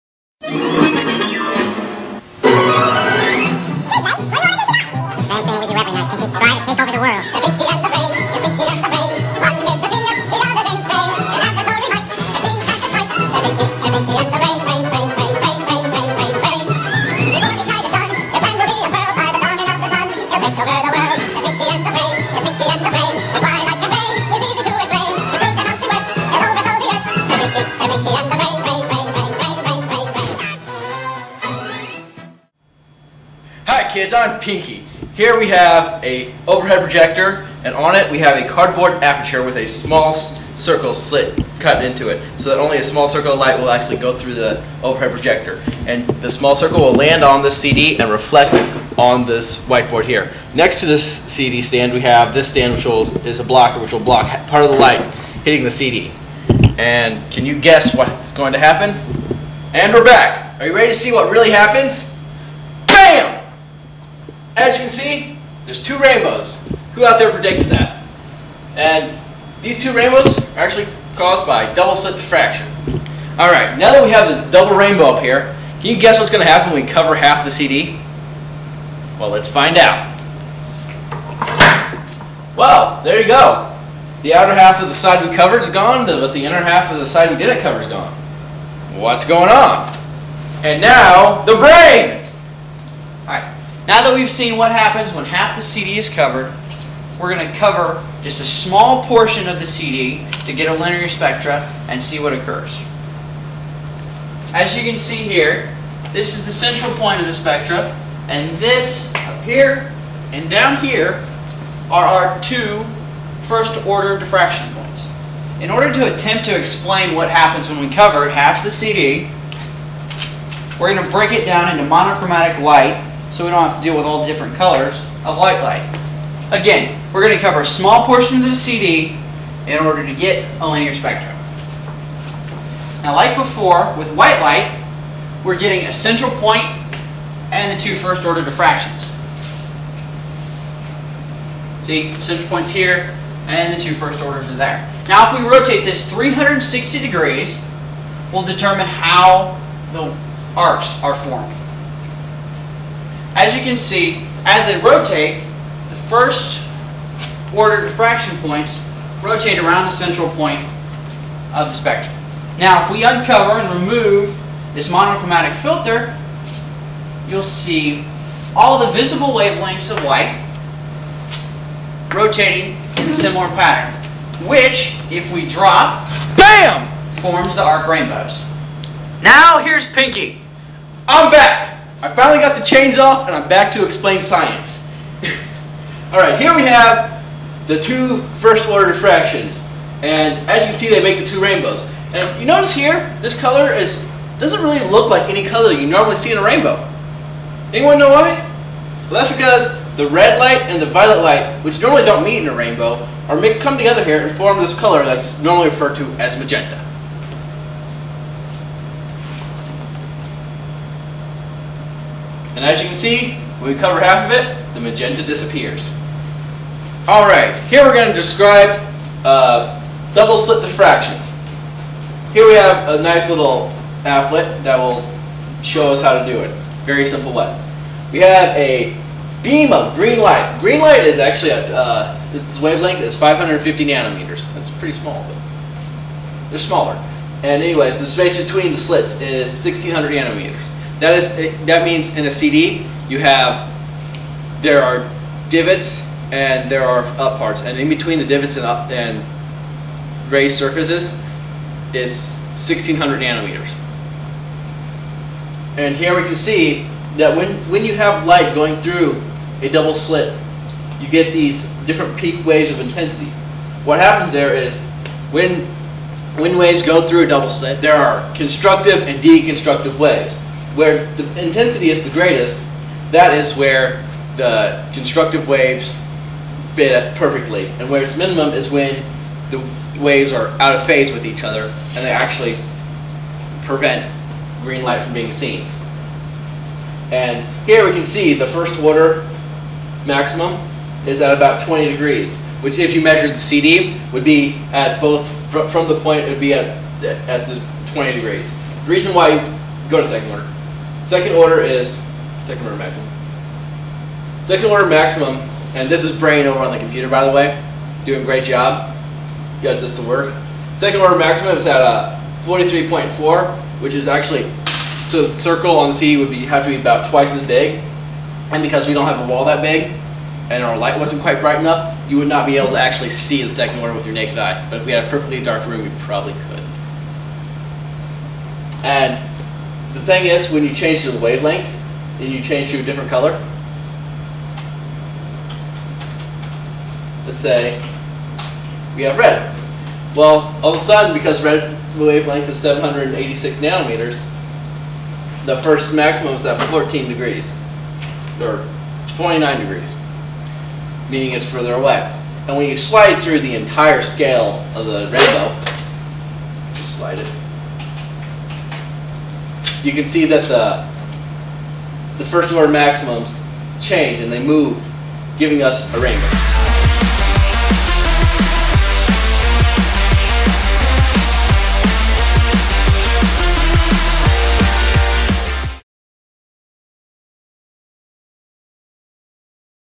Rockhurst University's All New Physics Review: CD Diffraction
Part of Rockhurst University's All New Physics Review: CD Diffraction presented by Alex Thorne & Paul Nickell